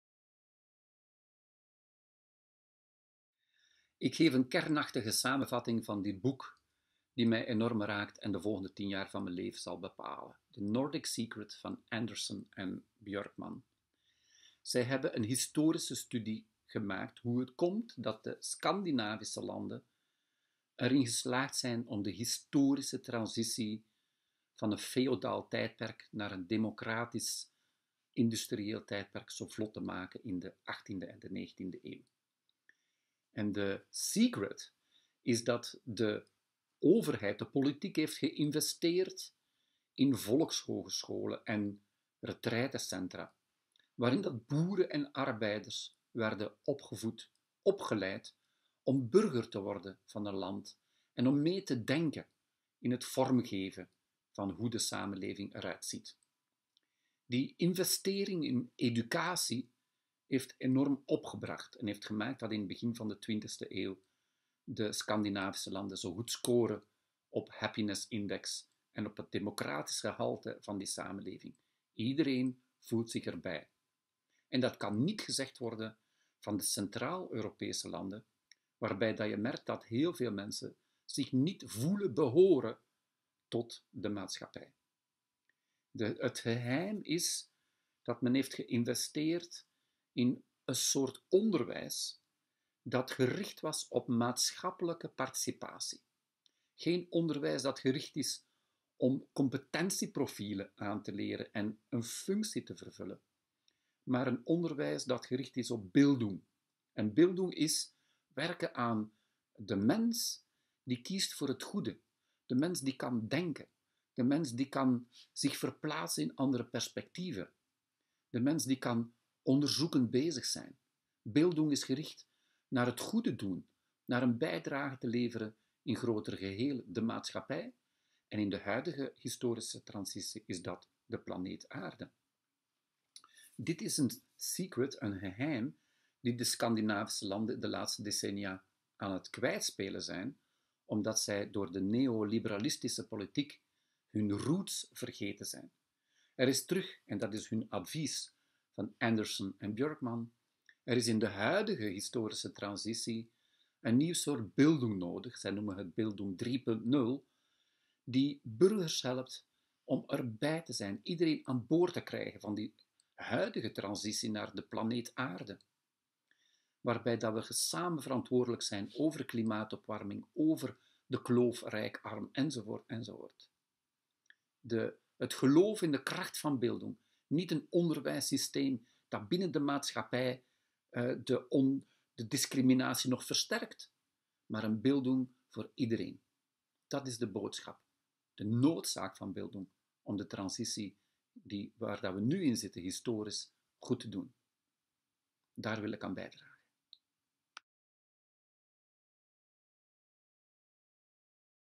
Bildung 3.0: persoonsvorming gericht op participatie
Anderson & Björkman legden het geheim bloot van het succes van de Skandinavische landen. Ze adviseren eenzelfde aanpak voor de huidige mondiale transitie naar een meer ecologische en rechtvaardige samenleving.